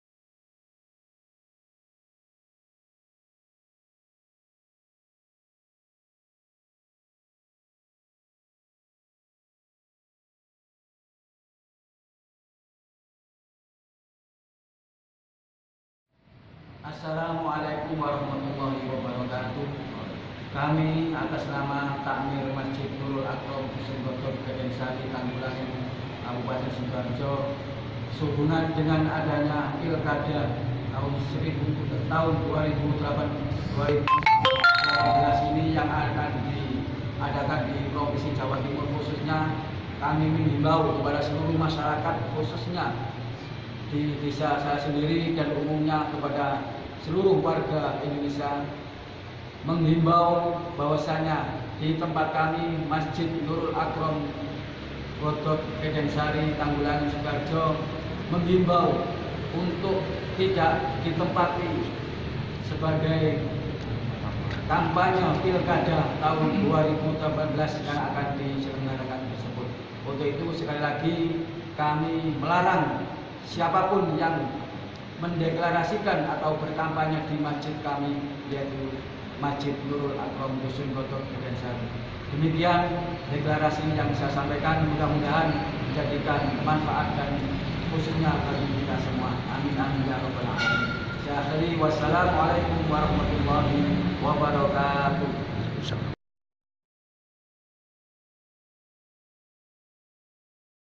DEKLARASI TAKMIR MASJID NURUL AKHROM TANGGULANGIN
TAKMIR MASJID NURUL AKROM DI DESA KEDEN SARI KEC TANGGULANGIN SIDOARJO MENOLAK MASJID DIJADIKAN TEMPAT BERPOLITIK